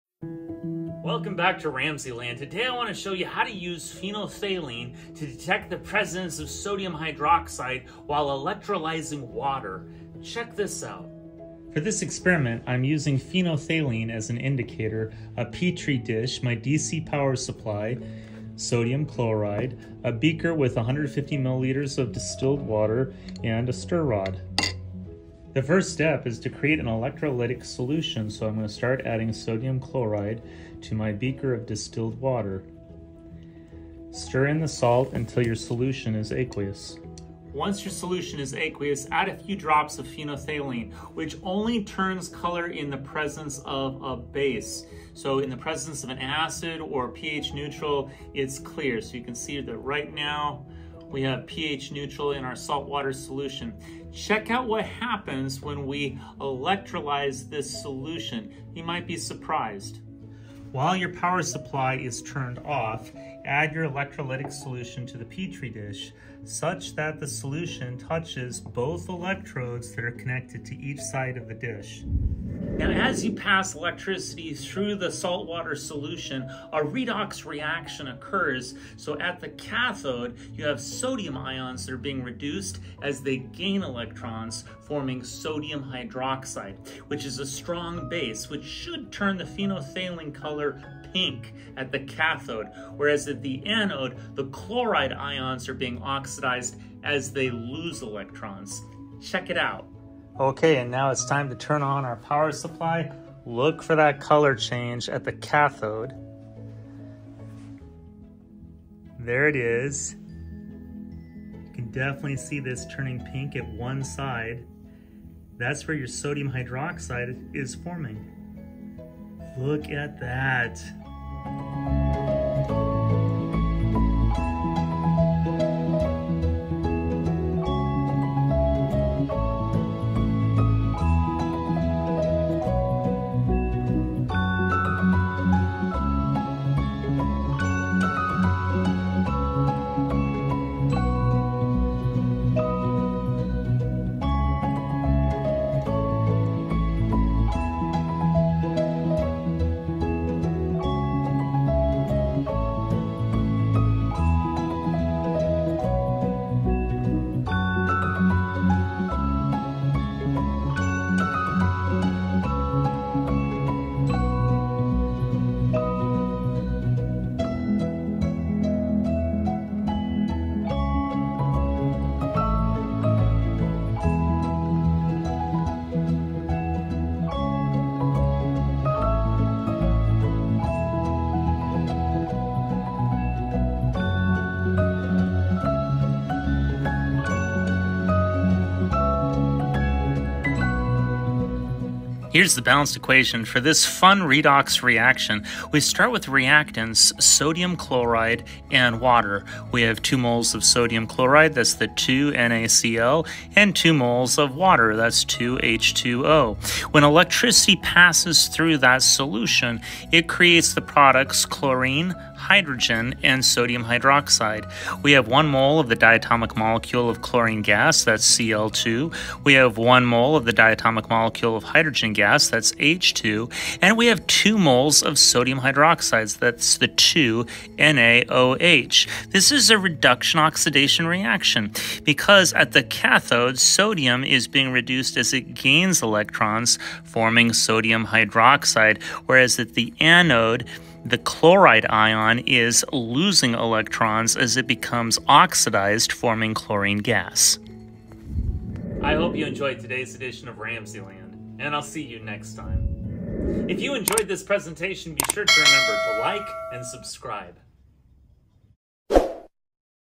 Electrolyze Saltwater with Phenolphthalein in RamZland!⚗️ 2NaCl+2H2O → CI2+H2+2NaOH
Keith Ramsey explains the electrolysis of saltwater in the presence of phenolphthalein. 2NaCl+2H2O → CI2+H2+2NaOH